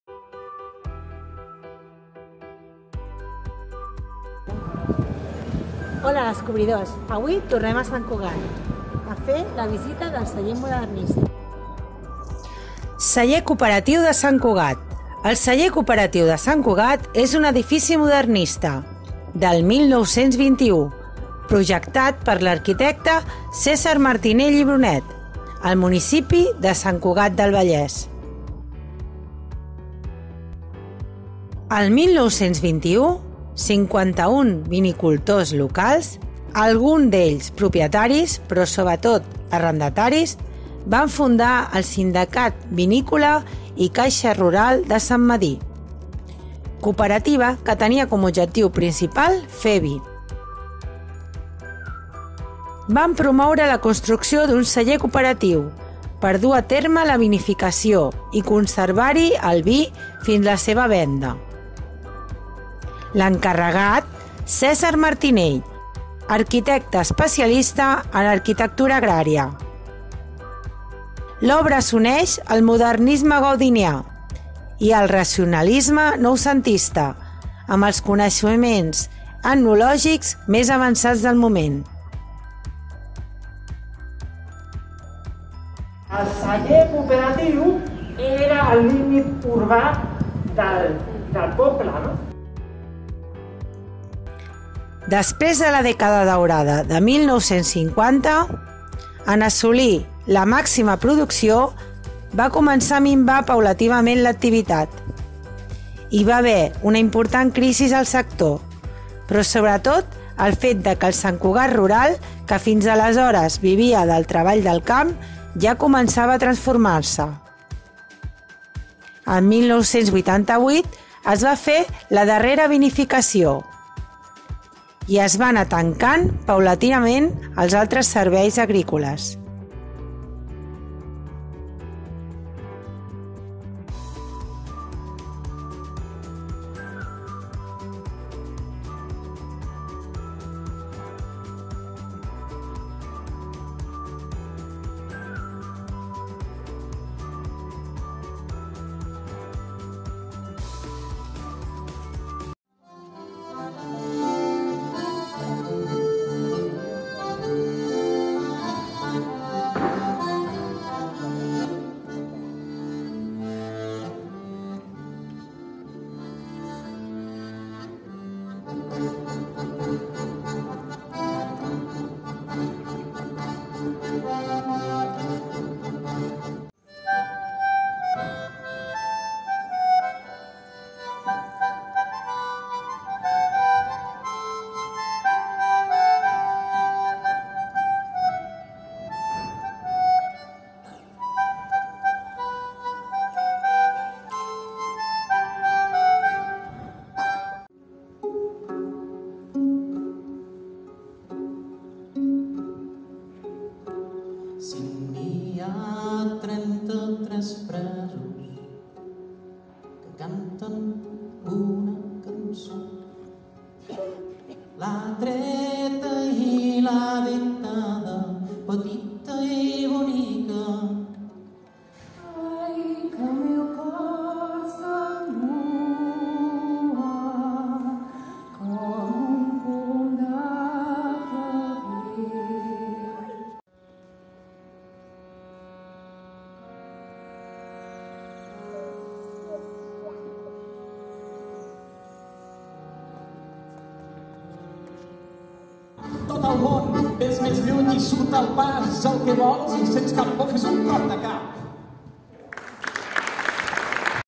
Sant Cugat (cap 3) Celler modernista
Cèsar Martinell, l’arquitecte especialista en arquitectura agrària. va construir el celler modernista de Sant Cugat.